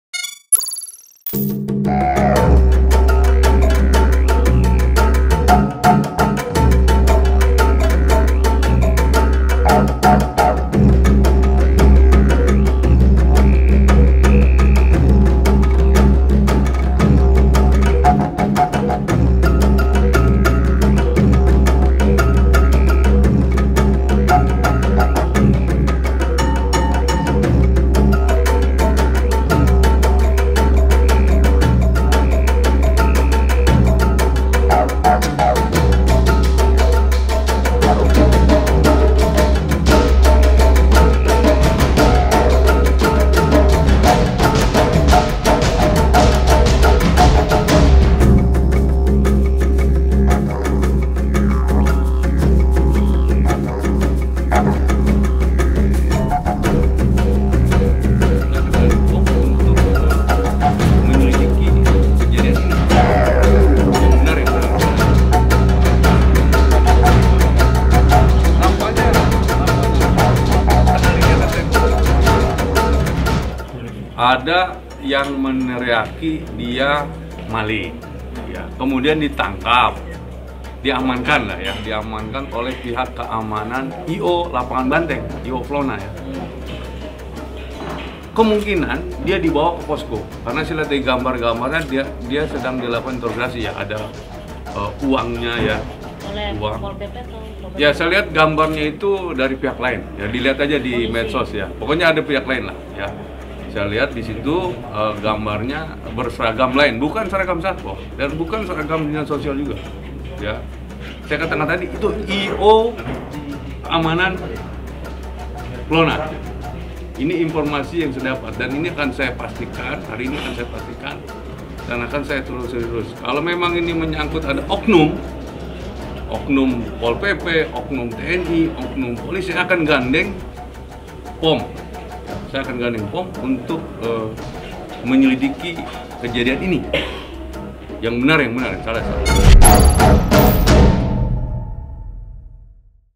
Satpol PP Bantah Aniaya Pria Gangguan Mental di Lapangan Banteng
Pria yang memiliki gangguan mental ditemukan dalam kondisi luka memar dan penuh sudutan rokok di setelah sempat hilang di Jakarta. Menanggapi hal itu Satpol PP DKI Jakarta mengaku pihaknya tidak terlibat dalam peristiwa penganiayaan itu.